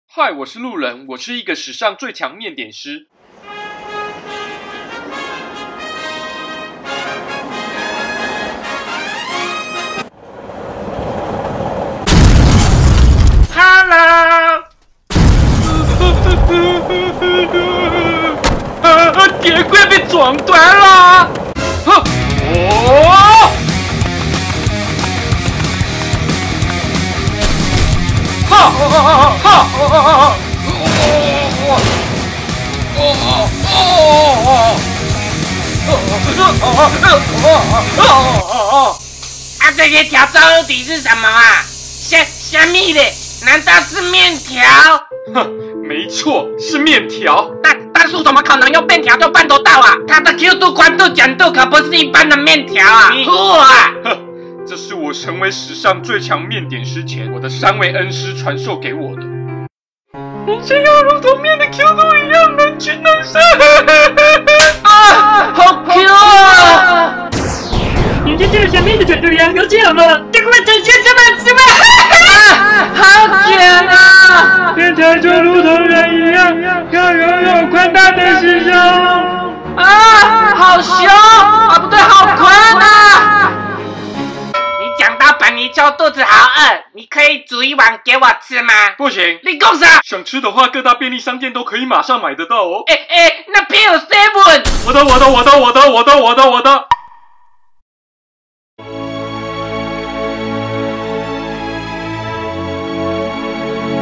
【巷口乾麵 ‖ 三麵人絕招大公開】自己以為很會救人的故事
滿載乘客的列車即將墜落斷軌
是何等強勁的條狀物，原來是巷口乾麵的麵條!!

巷口乾麵麵條寬、Q、捲，究竟是如何拯救生命!?